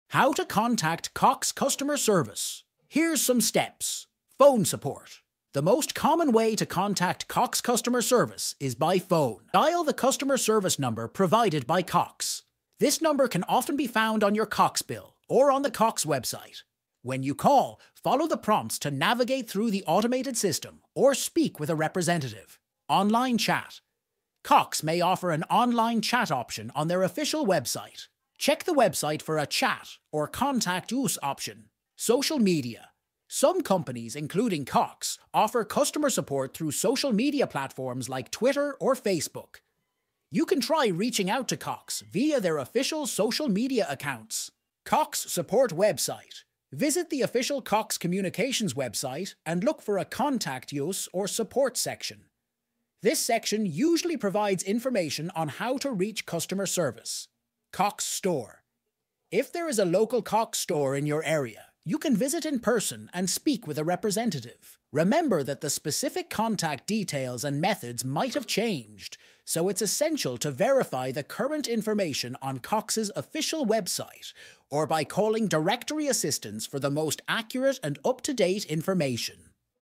How To Contact Cox Customer Service (2023)
00:00 Intro
00:05 Phone Support
00:22 Online Chat
00:31 Social Media
00:45 Cox Support Website